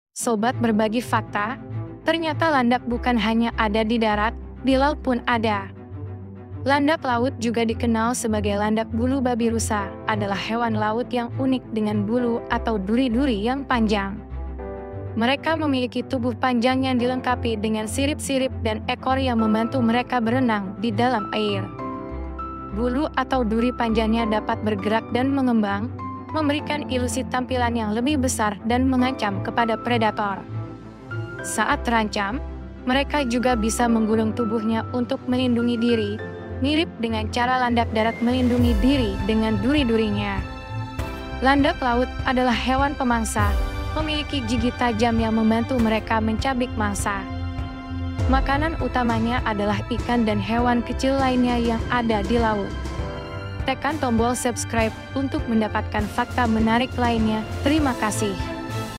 Fakta Unik Landak Laut
Landak Laut juga dikenal sebagai Landak Bulu Babirusa adalah hewan laut yang unik dengan bulu atau duri-duri yang panjang. Mereka memiliki tubuh panjang yang dilengkapi dengan sirip-sirip dan ekor yang membantu mereka berenang di dalam air.

Bulu atau duri panjangnya dapat bergerak dan mengembang, memberikan ilusi tampilan yang lebih besar dan mengancam kepada predator.

Saat terancam, mereka juga bisa menggulung tubuhnya untuk melindungi diri, mirip dengan cara landak darat melindungi diri dengan duri-durinya. Landak Laut adalah hewan pemangsa, memiliki gigi tajam yang membantu mereka mencabik mangsa.

Makanan utamanya adalah ikan dan hewan kecil lainnya yang ada di laut. Habitat utama landak laut adalah perairan tropis dan subtropis di berbagai wilayah dunia.

Meskipun tidak termasuk spesies yang sangat terancam punah, aktivitas perburuan dan kerusakan habitat laut tetap menjadi ancaman bagi kelangsungan hidup landak laut.

Upaya konservasi dan perlindungan laut sangat penting untuk memastikan populasi landak laut dan keberlanjutan ekosistem laut yang mereka tinggali.